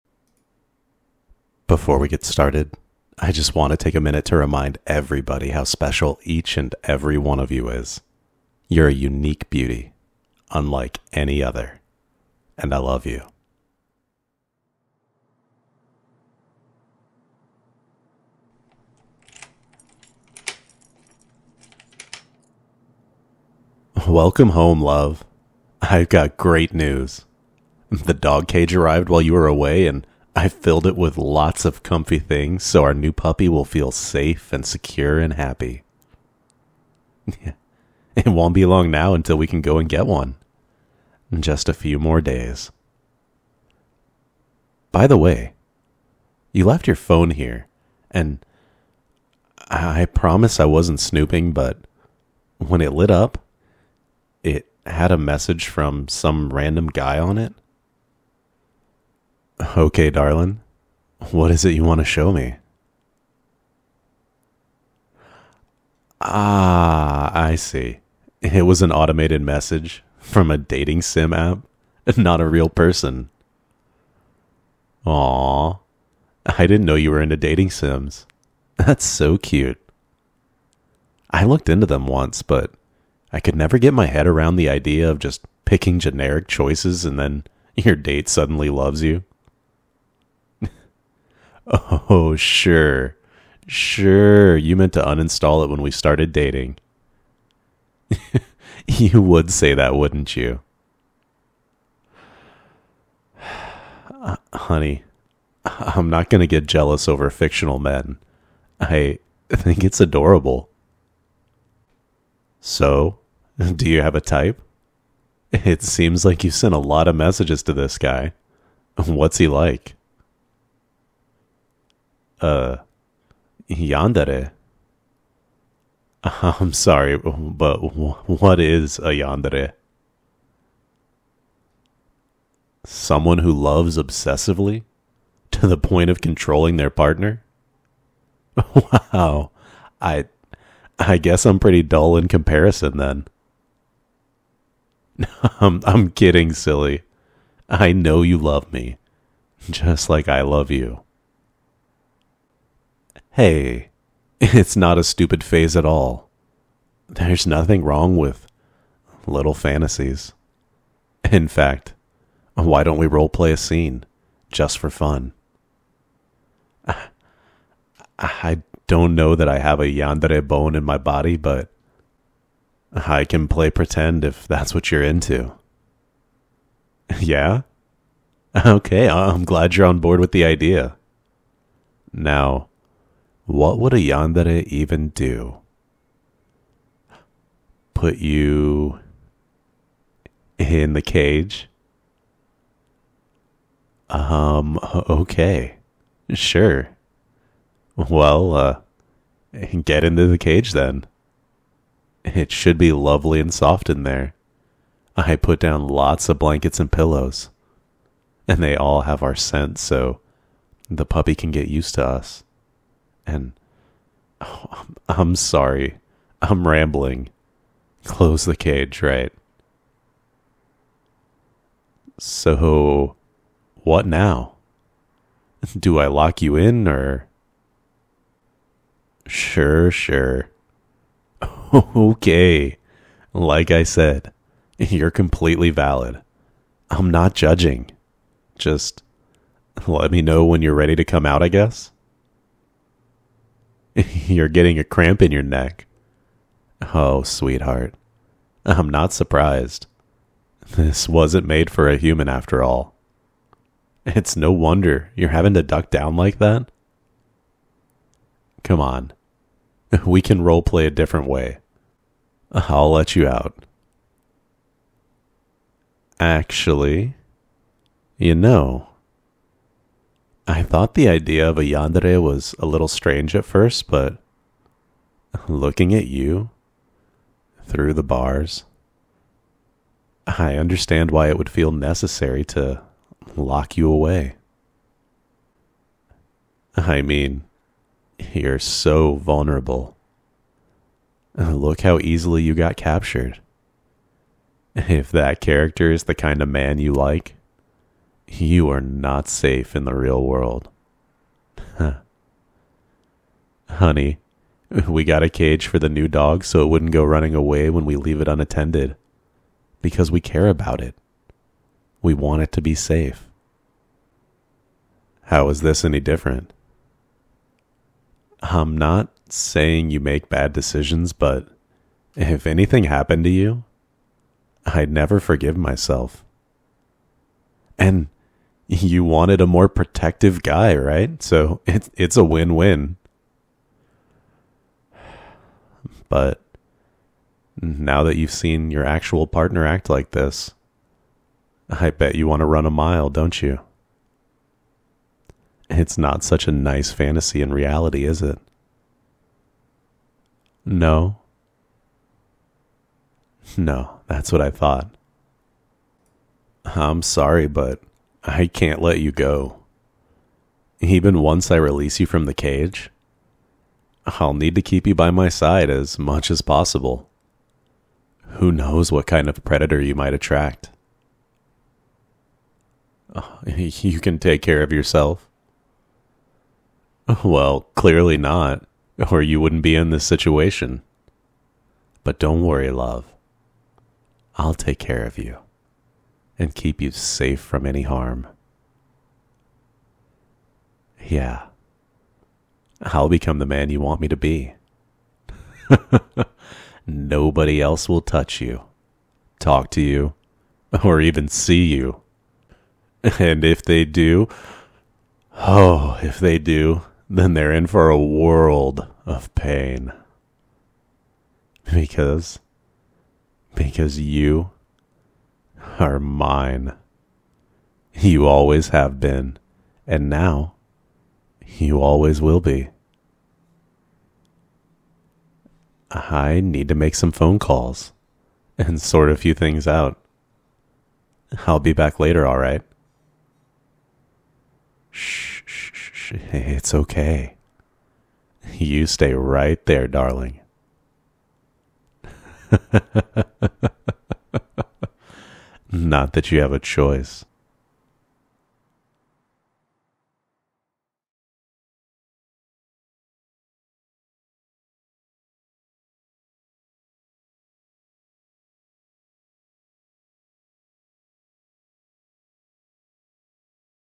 [M4A] Your Boyfriend Becomes An Accidental Yandere [SFW] [ASMR Roleplay]
Accidental Yandere - script written by: u/CelestialScripts

Setting: At Your Shared Place With Your Yandere

Tags: [Yandere Speaker] [Surprised Listener] [Flirting] [Kind of Wholesome] [Sleep Aid] [ASMR] [Boyfriend ASMR] [Vanilla] [Obsessive]

P.S. I did the thing again...I swear my brain just defaults to "you're" instead of "your"